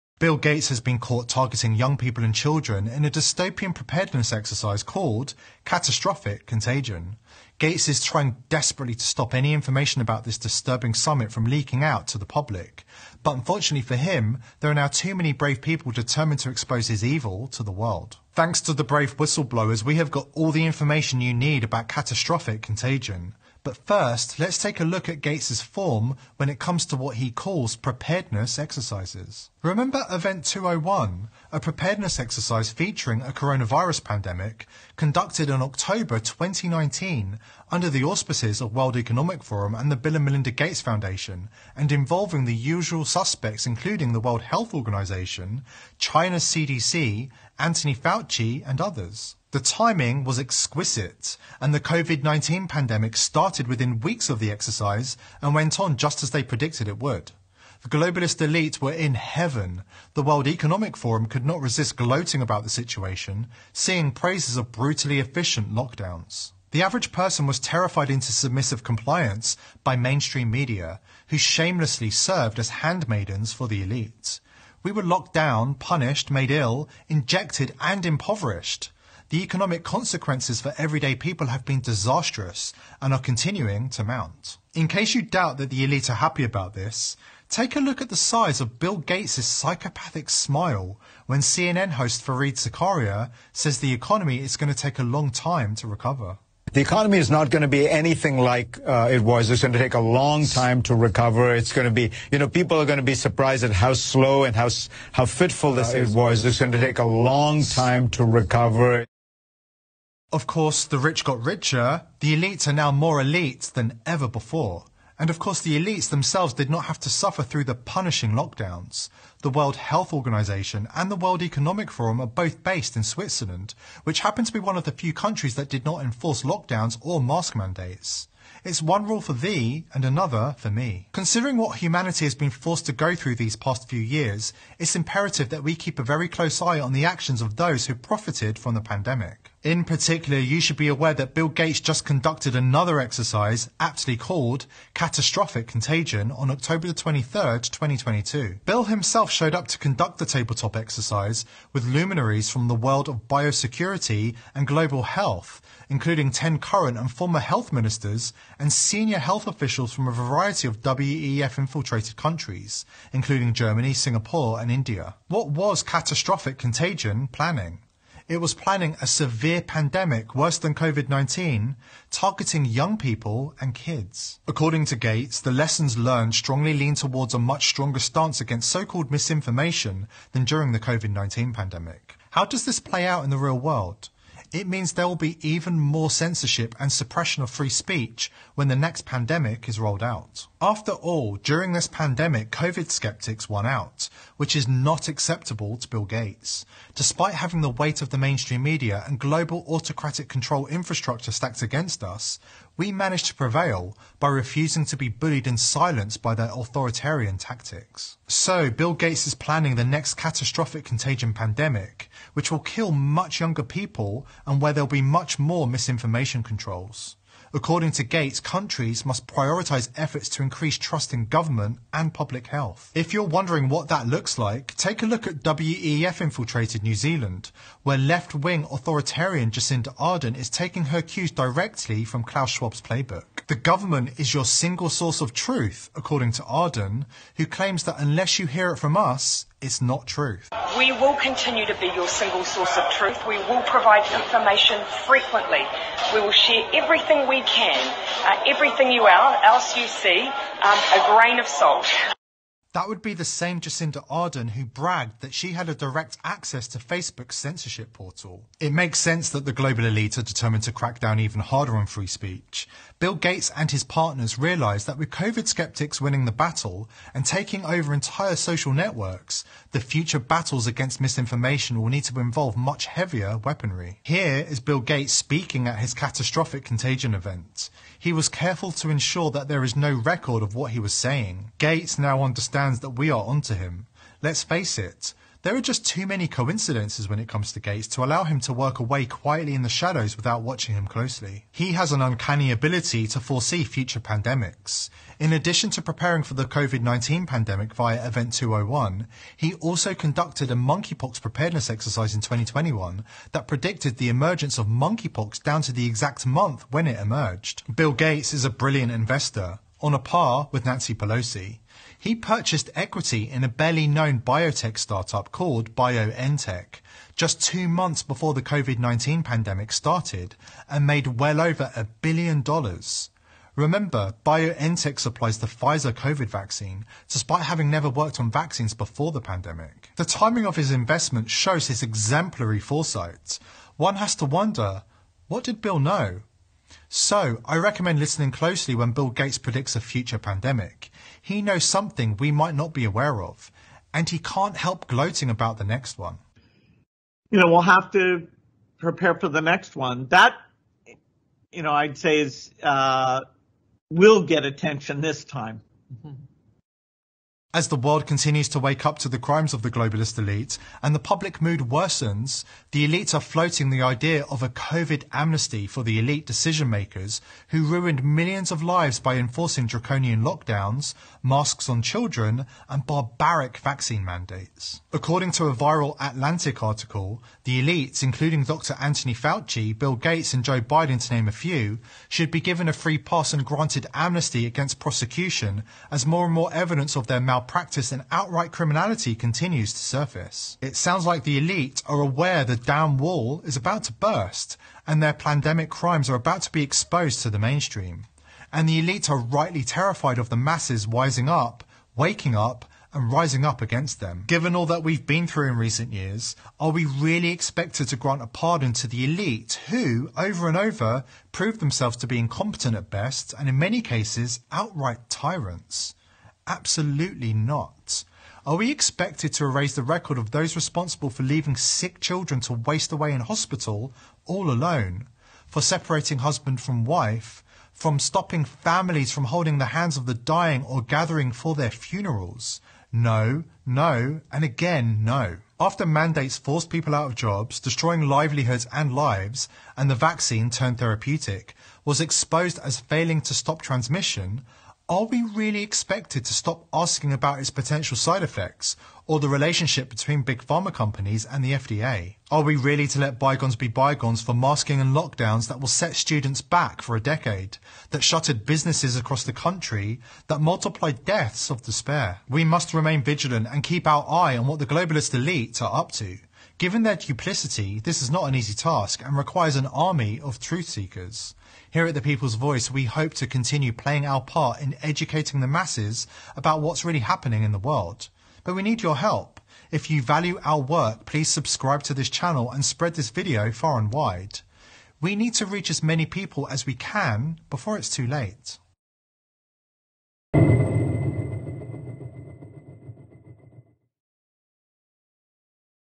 DISTOPIA PIANIFICATA ED APPLICATA
Video molto interessante in cui si mettono insieme diversi tasselli del mosaico della presunta pandemia. Il nostro amico Bill Gates, come sempre insieme ai 'soliti sospetti' si permette persino di ridere soddisfatto quando un giornalista gli elenca i danni, che tutte le misure sanitarie suggerite dai ""filantropi"" come lui, hanno fatto sulla nostra economia. Non riesce proprio a trattenersi, come un bimbo felice.